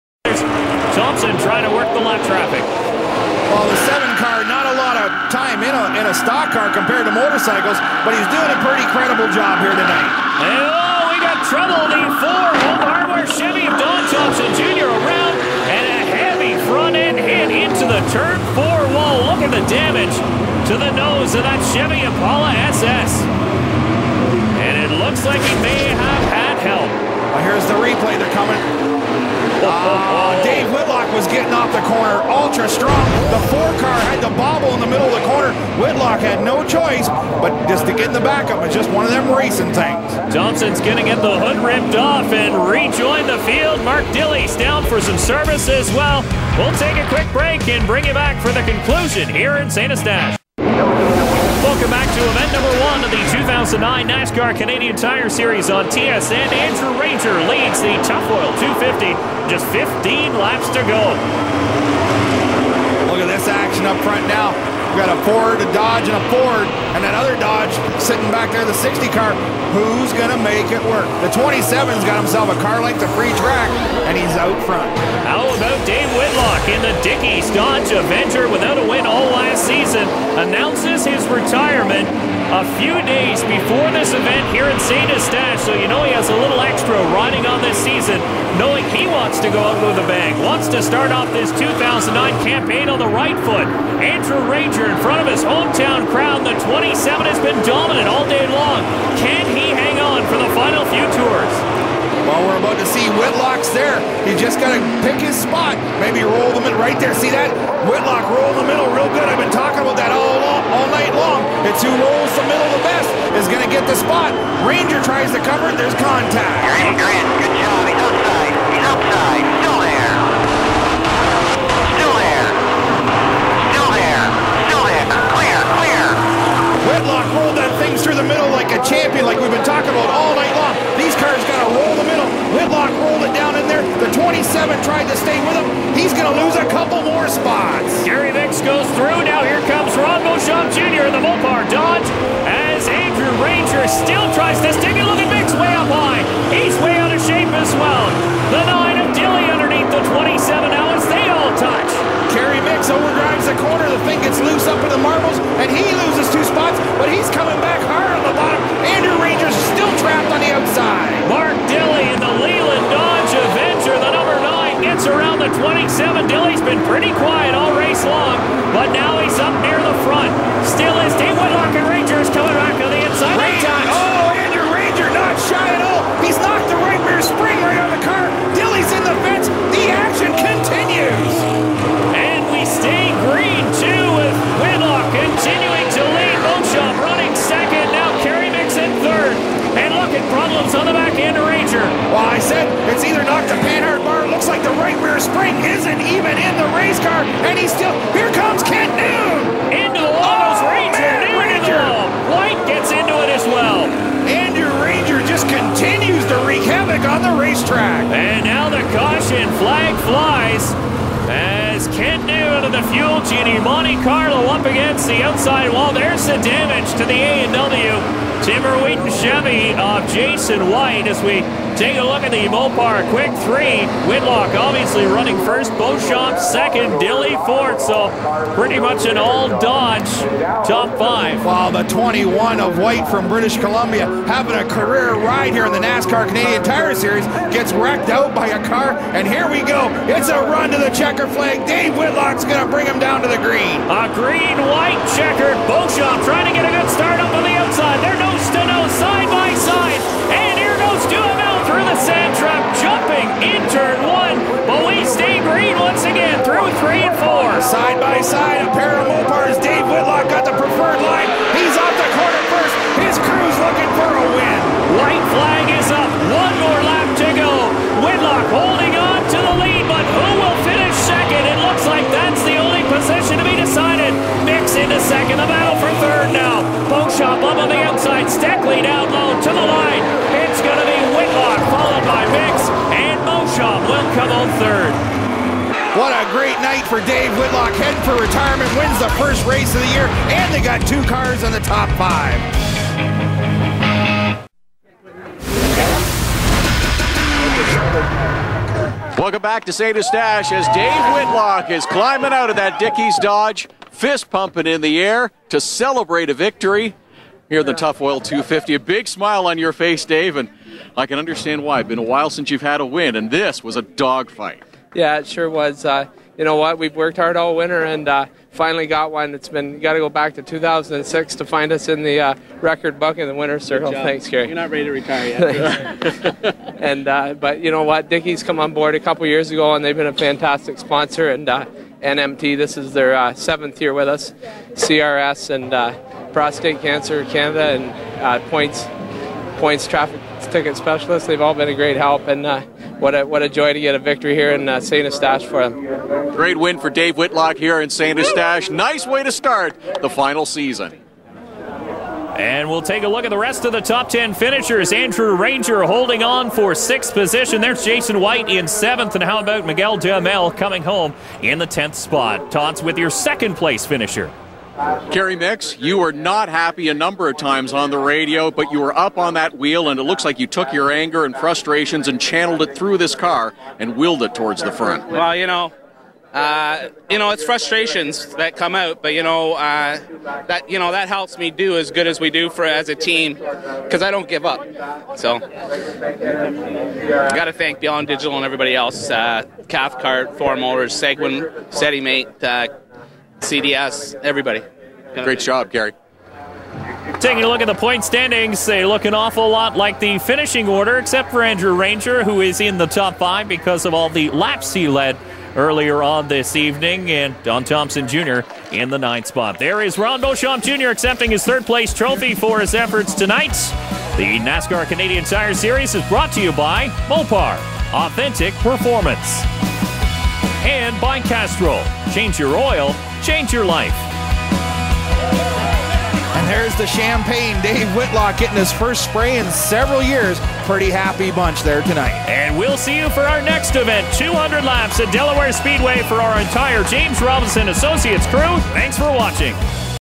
2009 Tufoil 250 - PART 5/5 (Finish/Post Race)
There is almost too much action to follow at once as we get down to the end of the Tufoil 250 as #4 Don Thompson Jr spins and hits the wall hard in turn 4.  On the restart Andrew Ranger who has appeared to have the best car for most of the night gets pressure from #39 Dave Whitlock.  #02 Kerry Micks and #60 Ron Beauchamp Jr throw there hat into the fray for the win.  Things get real crazy and the race needs to be settled with a green white checkered finish.

We also have post race coverage with interviews with our top finishers as well as the field run down and points standings after race 1.
A great way to start the 2009 Canadian Tire Series season.

2009 Tufoil 250
Autodrome St.Eustache